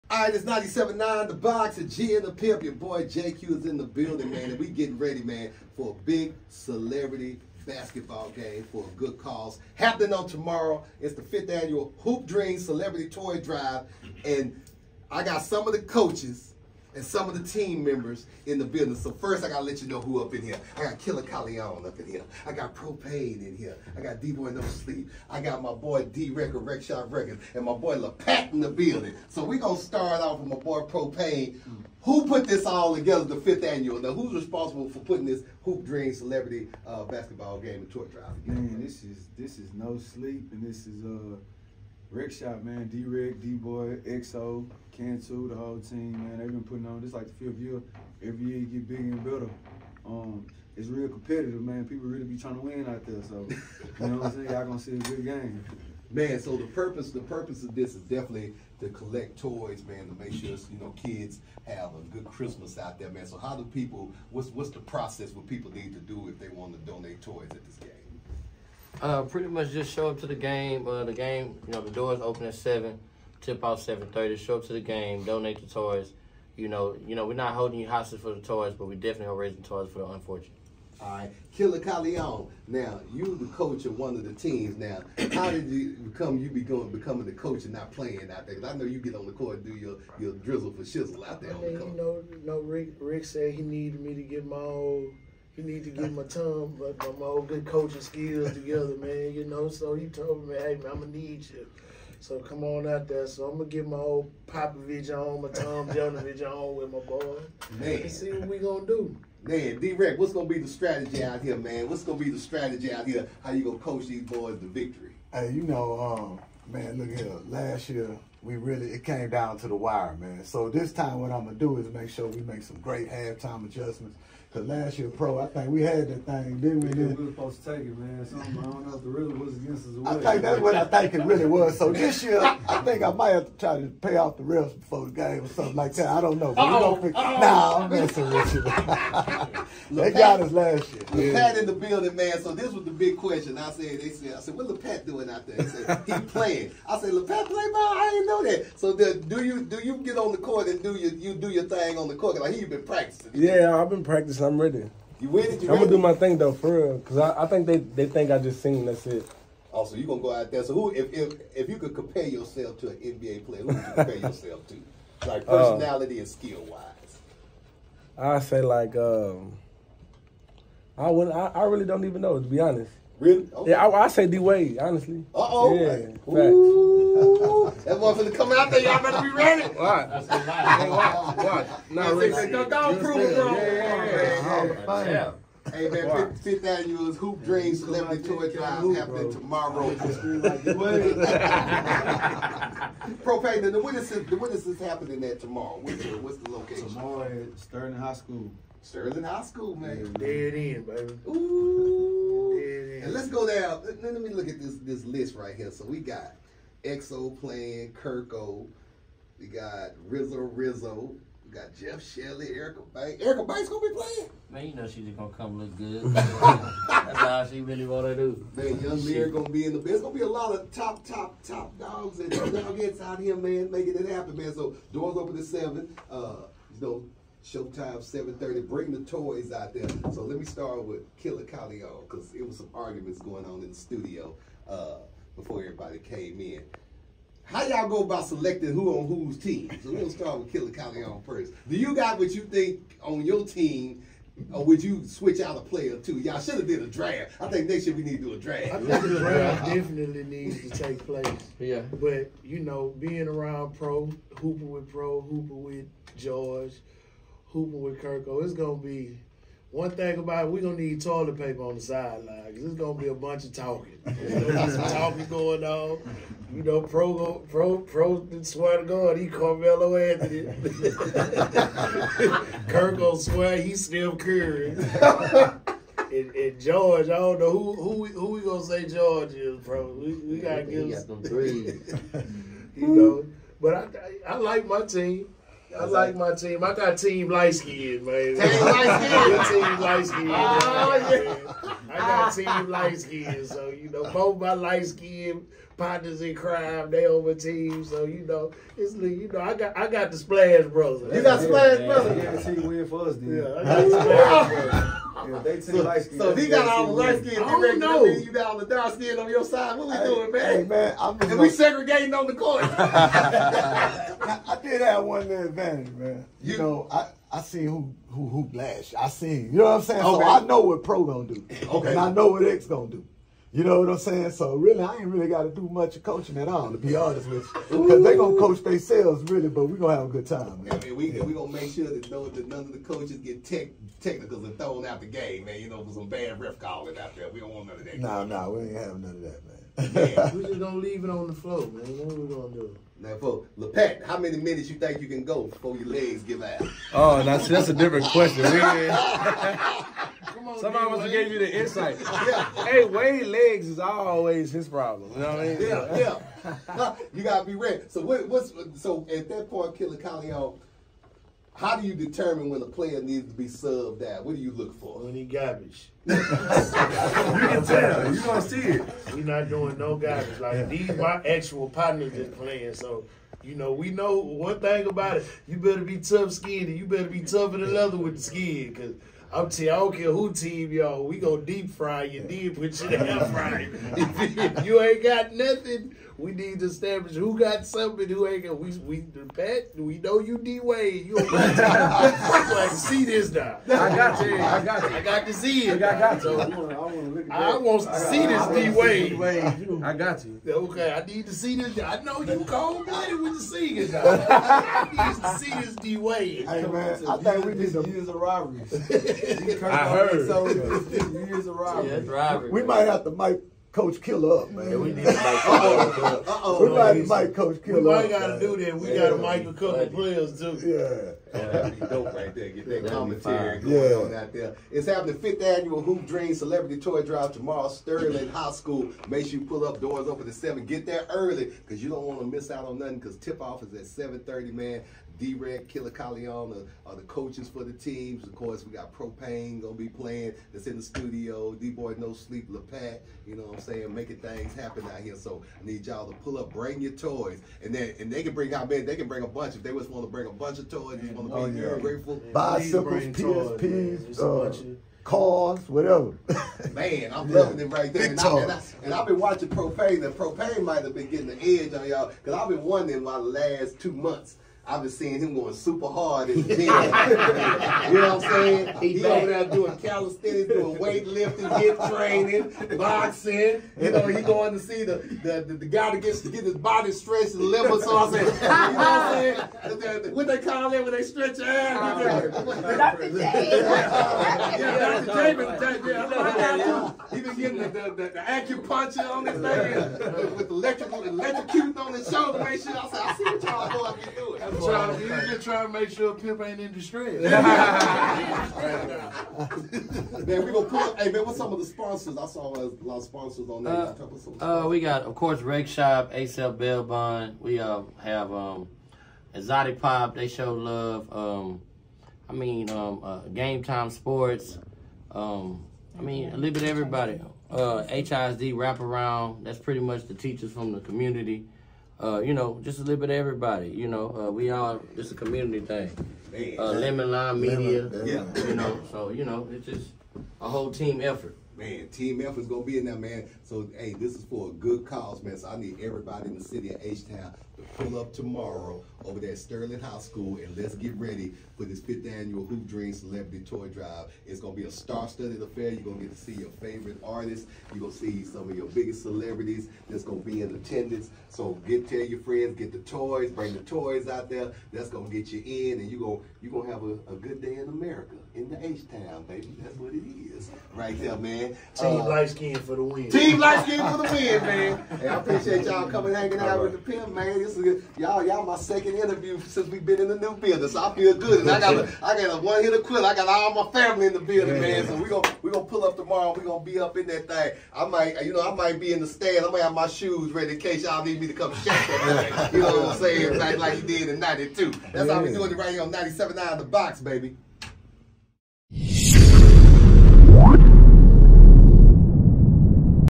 5th annual HOOP DREAMS Celebrity Basketball Game w/ Propain, Lah Pat, Killa Kyleon, D-Reck & more!
It's all going down Wednesday, December 22 at Sterling high school! ￼NoSleepMG WreckshopRecords ExoticPop ￼